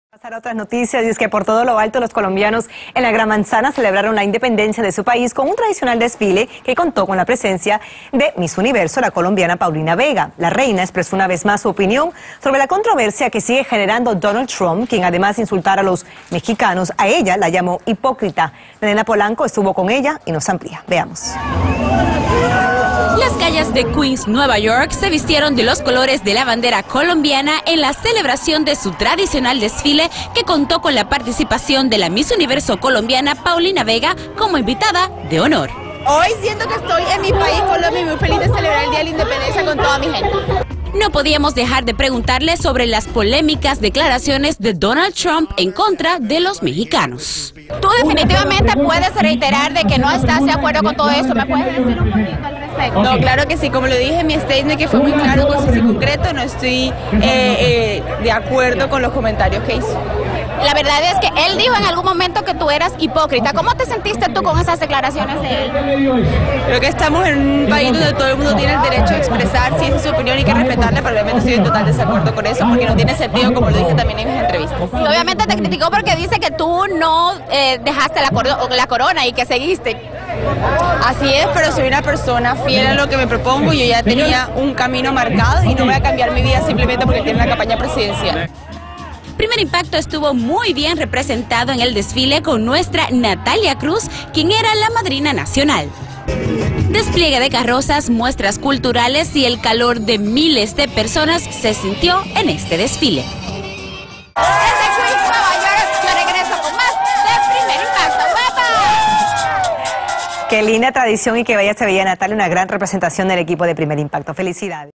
Paulina Vega, Miss Universo 2014 en entrevista para primer impacto
Paulina Vega, Miss Universo 2014 entrevistada por primer impacto durante el desfile de colombianos en Nueva York.

La Miss Universo reiteró sus declaraciones en contra de Donald Trump.